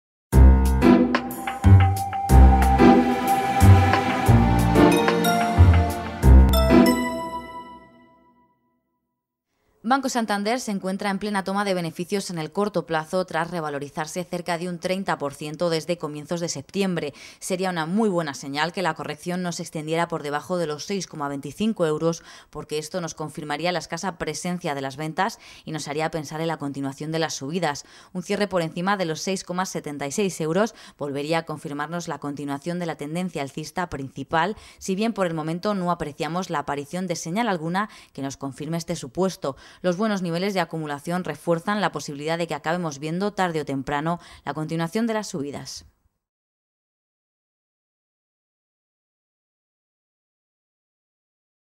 Análisis técnico de Banco Santander
Si la corrección no se extiende por debajo de los 6,25 euros, podremos pensar en la continuación de las subidas.